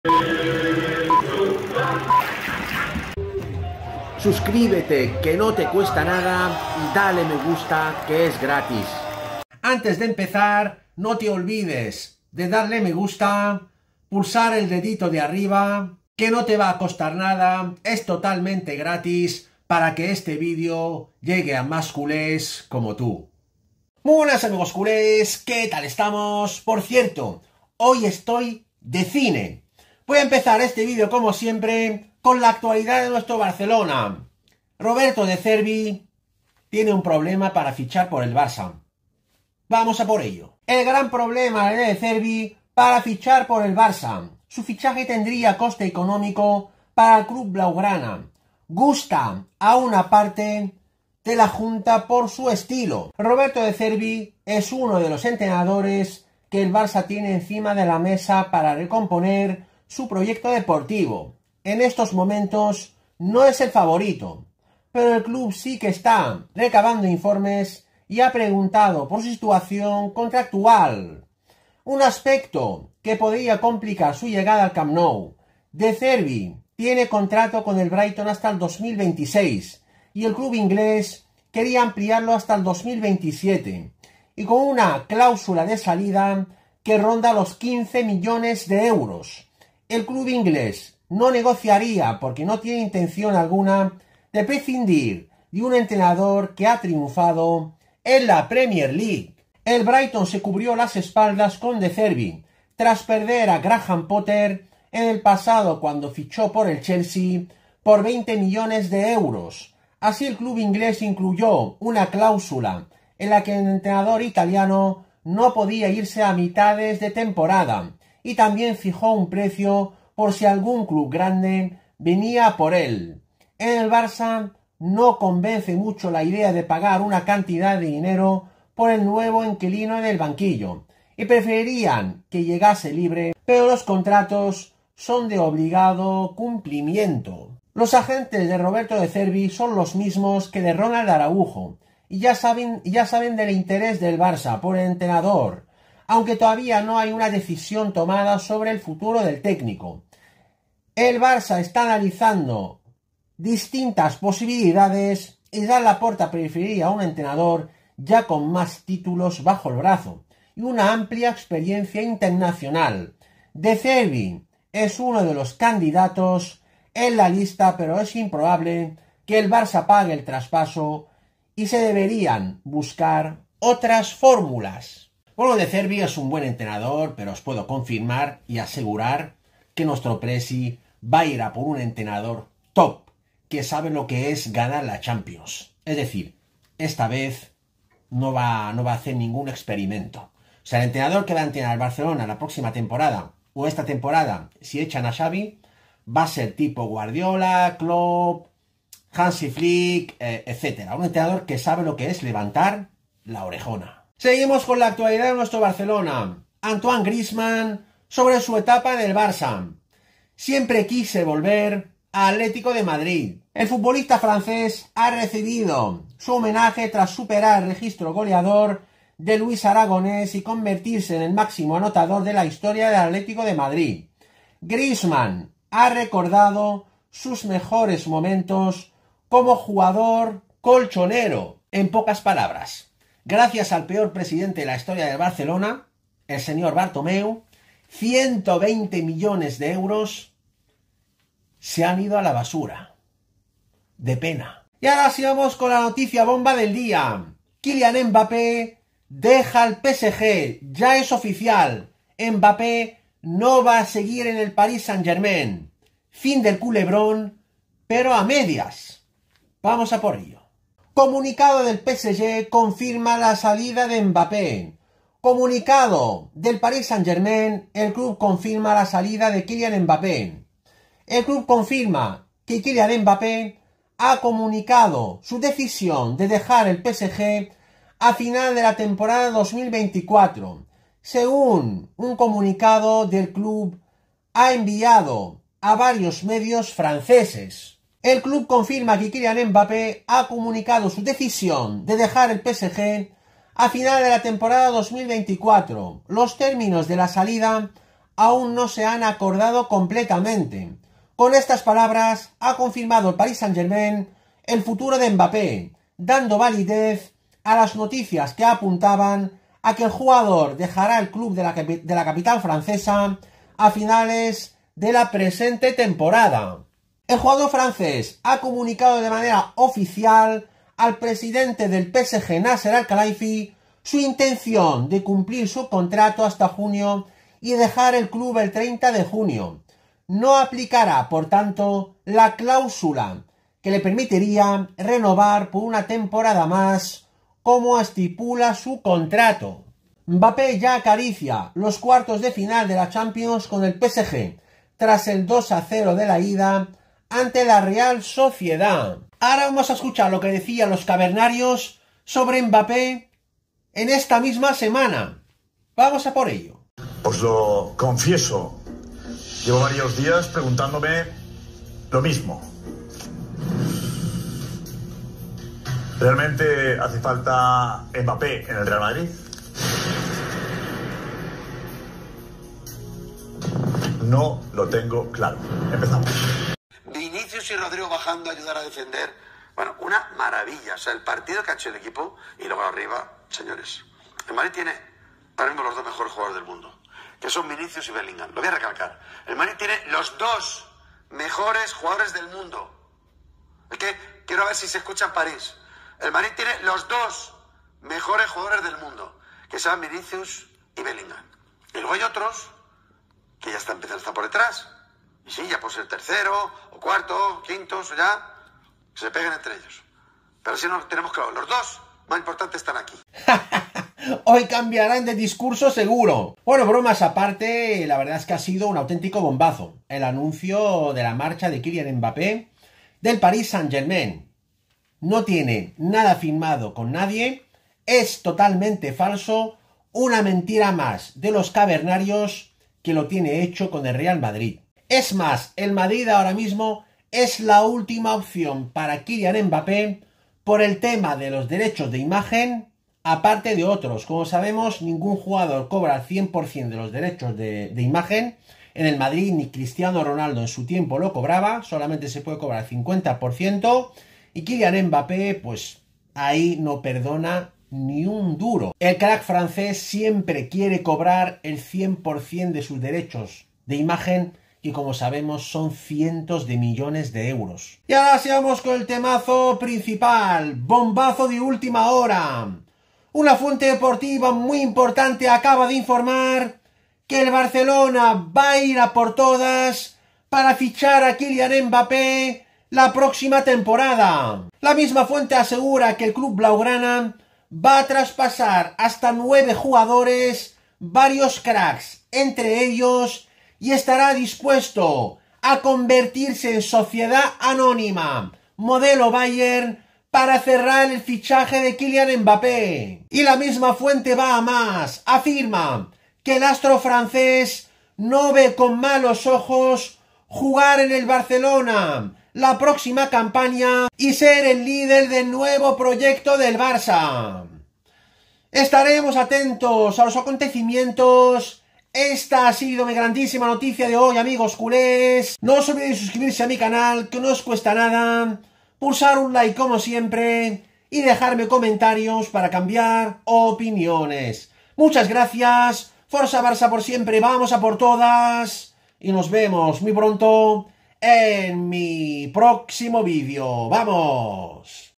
ÚLTIMA HORA ¡KYLIAN MBAPPÉ ACABA DE ANUNCIAR EL BOMBAZO UNIVERSAL DEL BARÇA!
Tras el anuncio bomba de este jueves con la salida ya confirmada de Kylian Mbappé del Psg en junio de este año 2024, una fuente deportiva muy importante asegura que el Barcelona va ir a por todas para fichar a Kylian Mbappé y esta dispuesto a hacer una revolución deportiva y institucional histórica, para que Kylian Mbappé sea nuevo jugador del Barça la próxima temporada